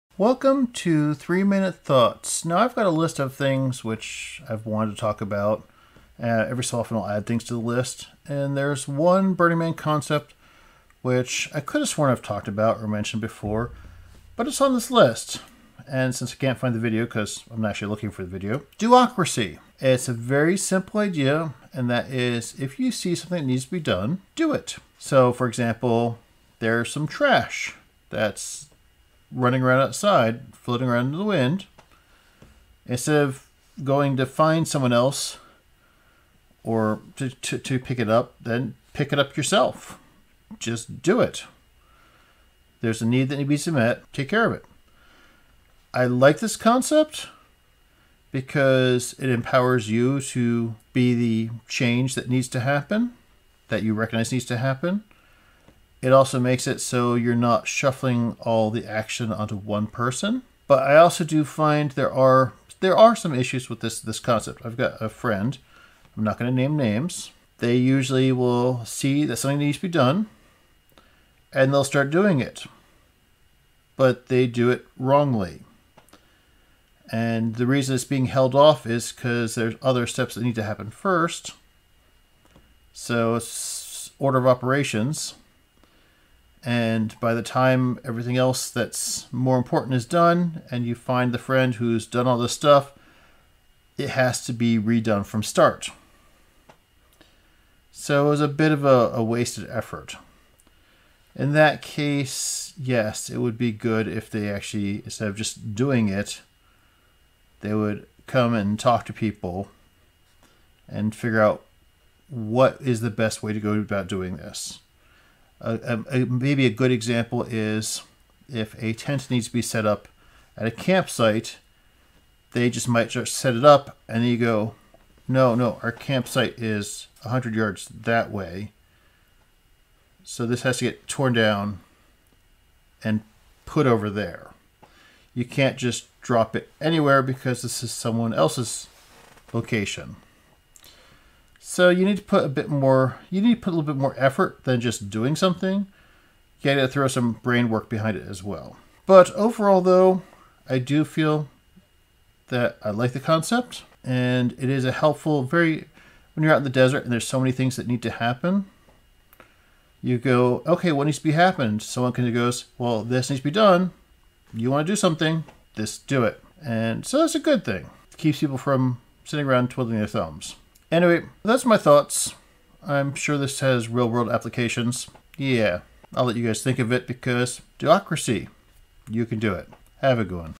What is a Do-ocracy
While not specifically a Burning Man term, it is one that is used frequently in the desert.